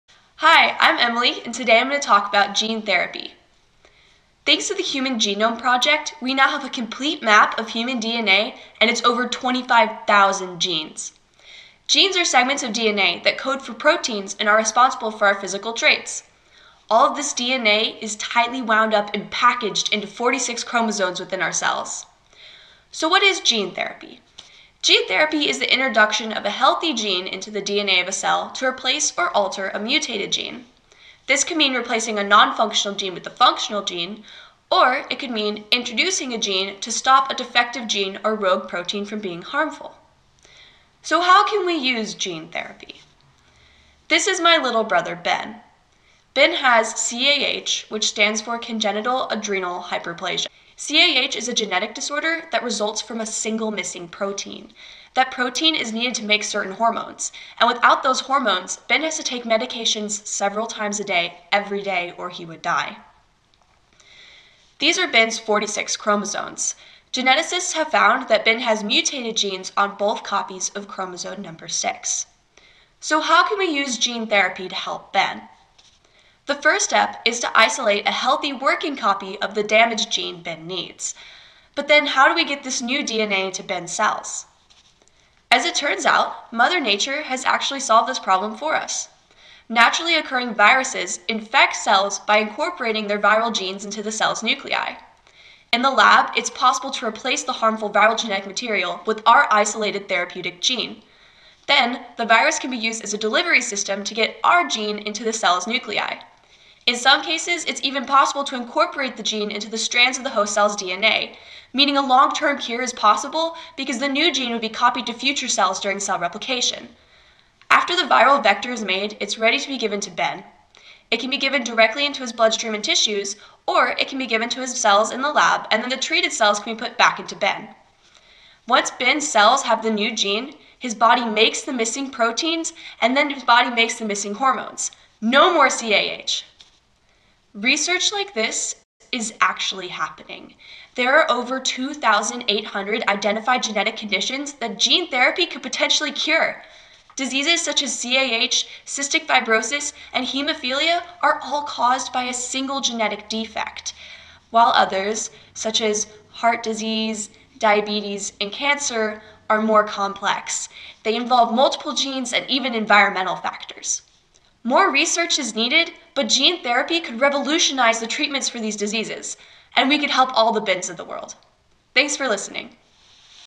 Gene Therapy and CAH- Science Ambassador Scholarship
Hi! My name is Emily Wilson and I'm a high school senior from San Antonio. I will be attending Princeton University next year, and plan on majoring in chemical and biological engineering. This video is intended to be a basic overview of gene therapy and how it can potentially revolutionize the medical field. One in ten people will develop an illness with a genetic component, just like my younger brother Ben. By choosing this topic I hope to spark interest in this rapidly developing field because more researchers are needed!

Made with the app VideoScribe.